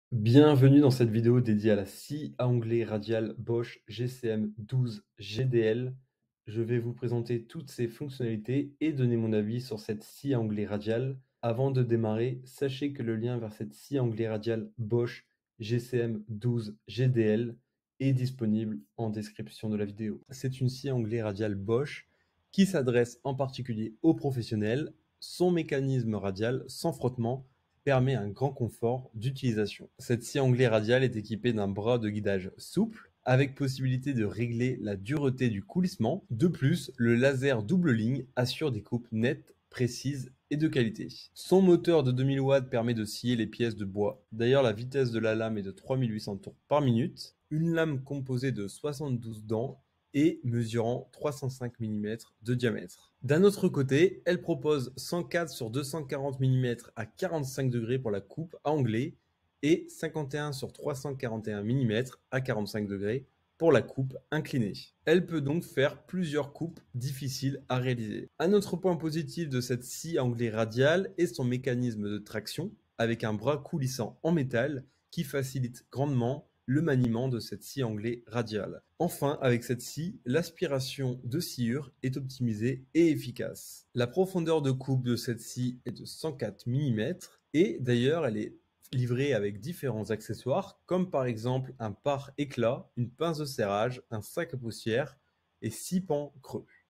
Scie à Onglet Radiale Bosch GCM 12 GDL : Mon avis complet
⭐ Scie à onglet radiale Bosch

Dans cette vidéo je fais une présentation complète de la Scie à Onglet Radiale Bosch GCM 12 GDL

Le juste choix participe au programme Amazon Partenaires.